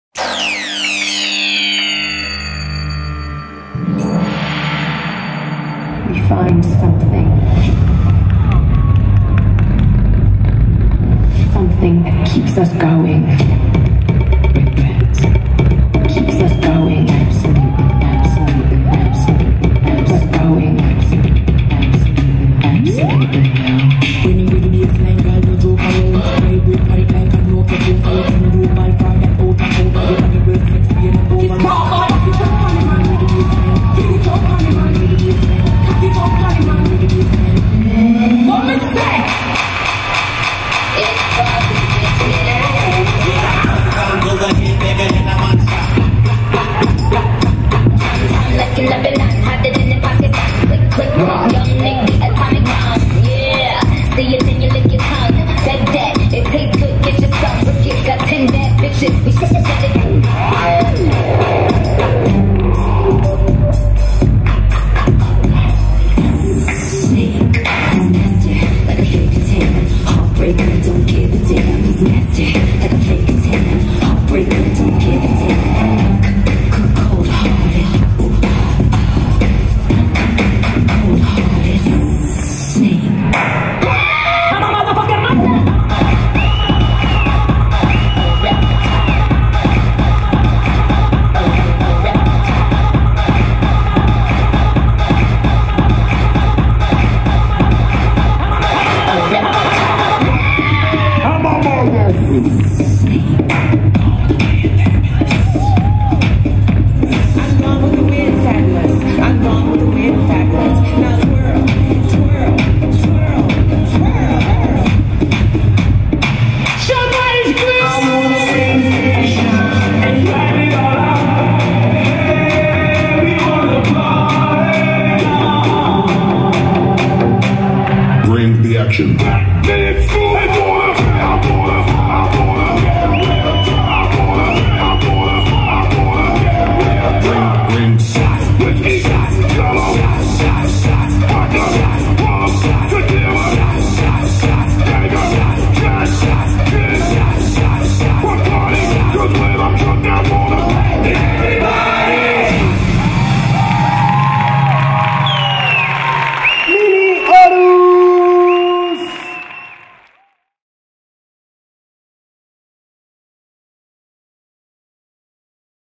#3 Infantil - Mini Horus |The Show Box 2014| HD
Mini Horus
Christian Castellanos

3er Clasificado Categoría Infantil
Campeonato de Danza Urbana The Show Box